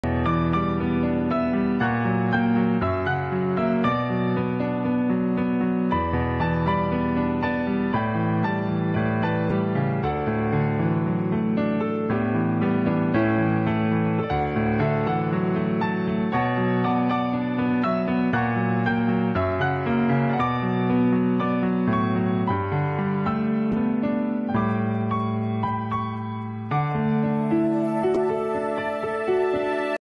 The Perseid meteor shower / shooting stars from the world's highest observatory in Tibet,China.
Good luck to you who see this video. And nice weekend! 😉😉